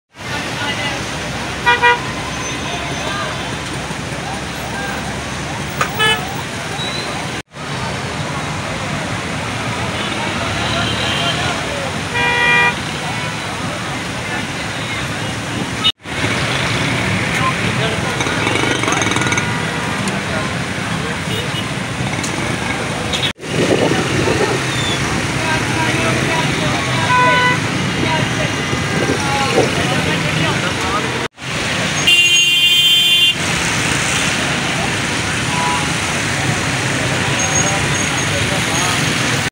Nagan Chowrangi Intermittent light to heavy rains continued in different parts of the city
Daily Footage Archive – Preserving Traditions, One Footage at a Time